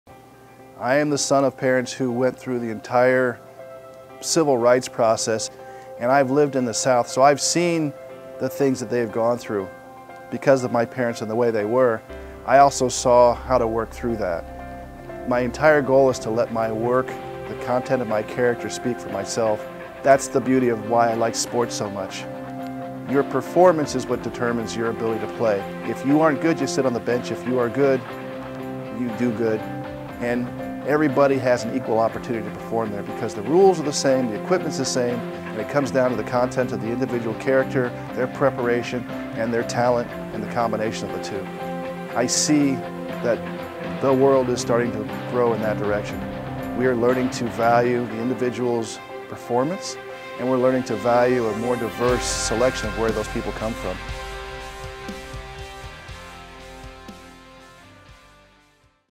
Profiles in Leadership: Rear Adm. John Fuller
To achieve optimal mission readiness, we provide every U.S. Navy Sailor and civilian with equal access to the tools and resources they need to succeed.

Listen as Rear Adm. John Fuller shares why his entire goal is to let his work and the content of his character speak for itself.

(U.S. Navy video/Released)